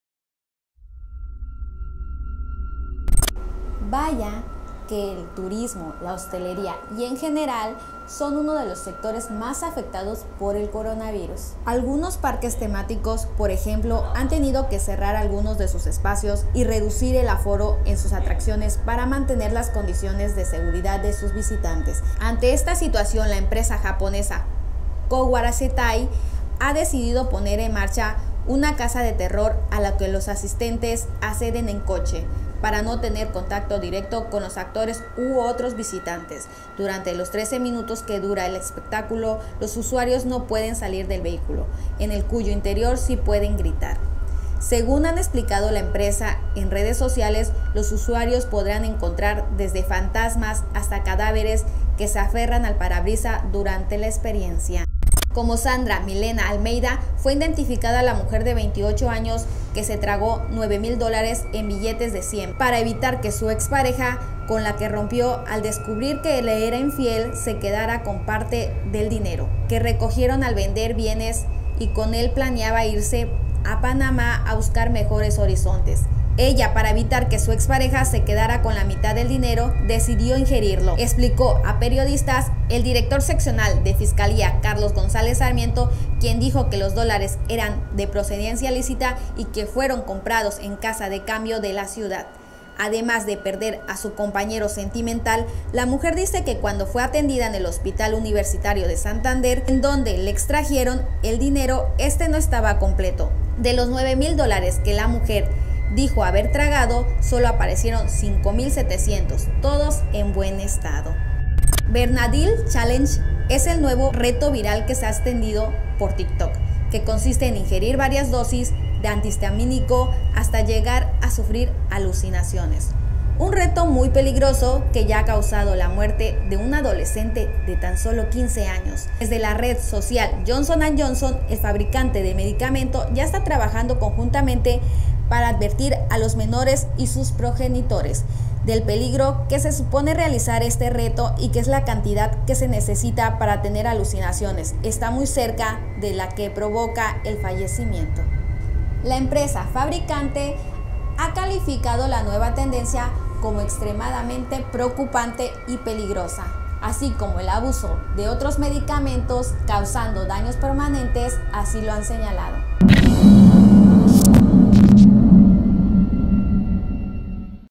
NOTAS PARA TOMARSE ENSERIO
1- Crean una casa del terror que se visita en coche para evitar los
2- Mujer se traga 9 mil dólares luego de pelear con su pareja
3- Nuevo reto causa la muerte de un joven